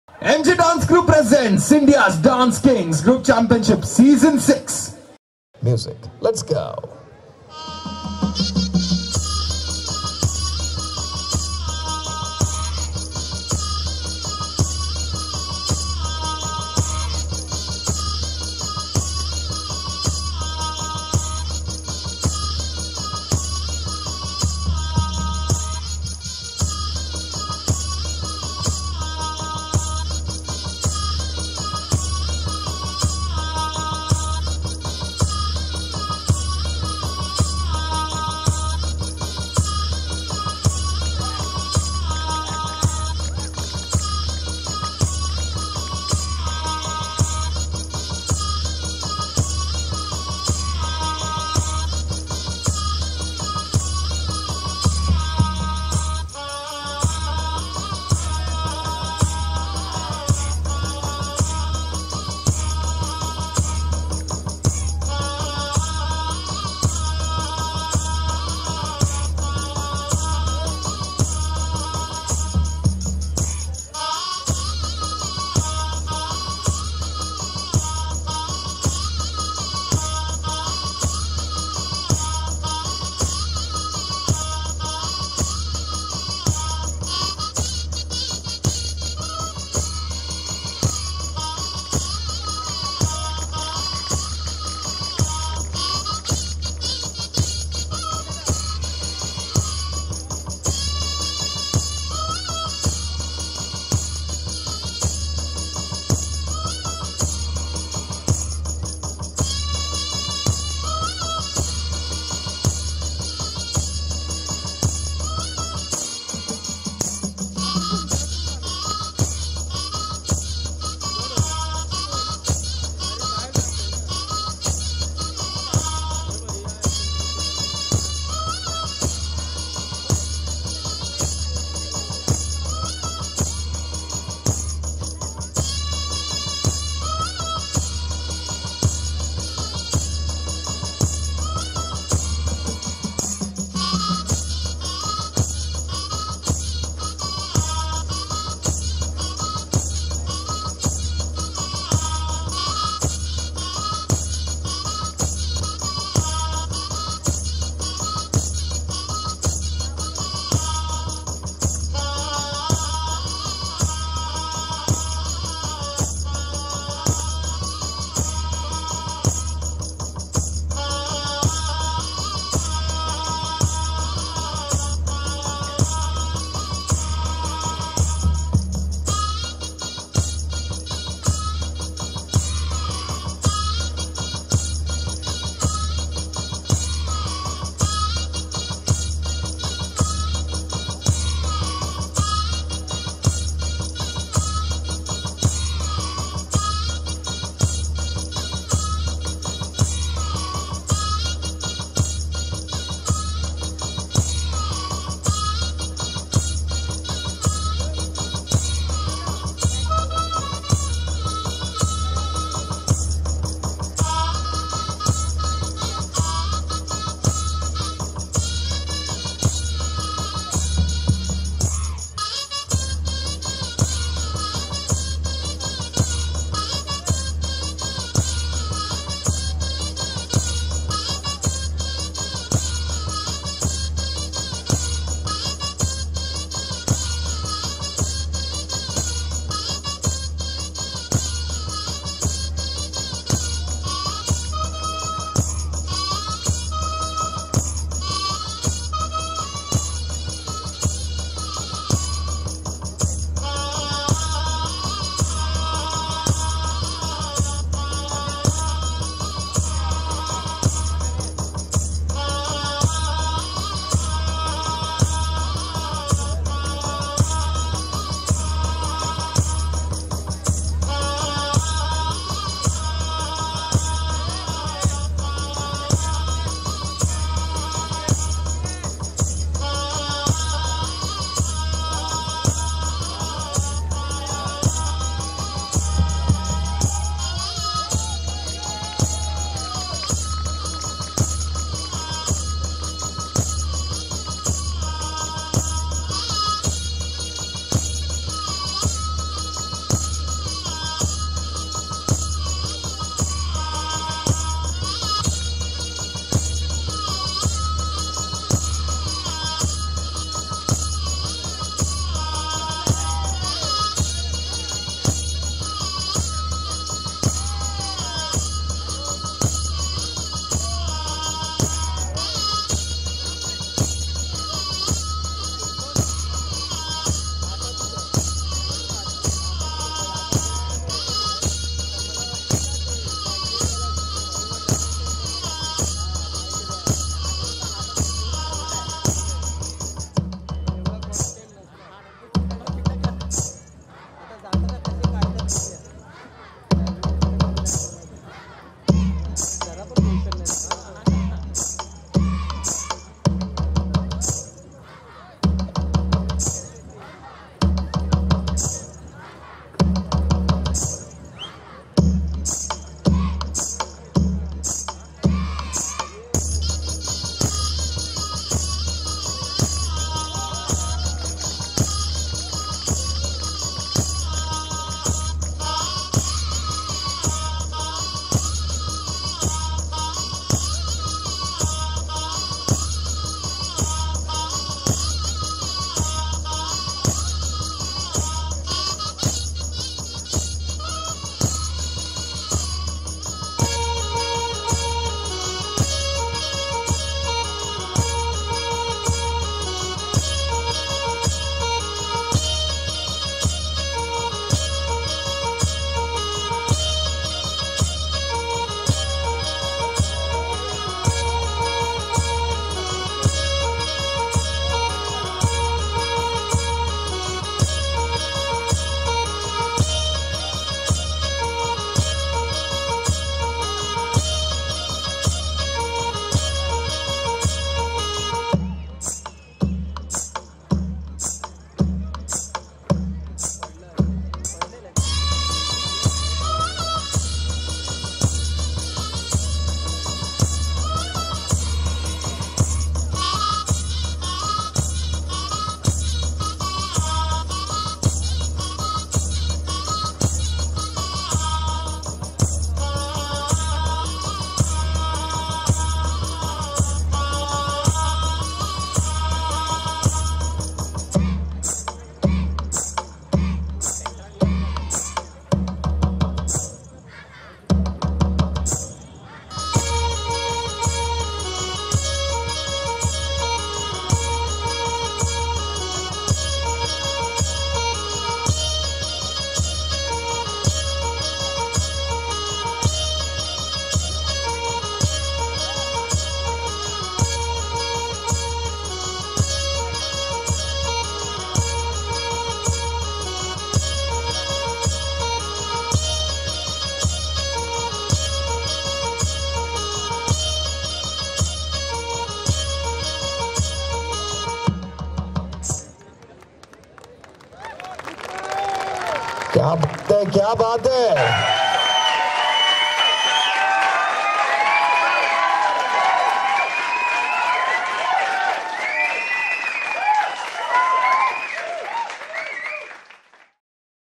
Vrajlal Parekh Vidyanidhi-Mumbai-Indias Dance Kings 2020 Season 6
MJ DANCE CREW NEW MUMBAI PRESENT
INDIA'S DANCE KINGS 2020-Season-6
SINCE-2008

YouTube-Family Of Indias Dance Kings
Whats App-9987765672